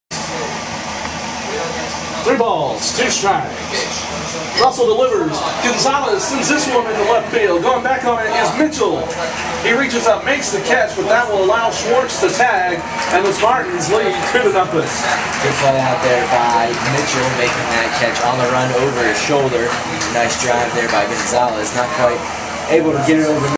Devin Gonzalez Sacrifice Fly
University of Tampa baseball player Devin Gonzalez hits a sacrifice fly against #4 Florida Southern on 04/23/2010.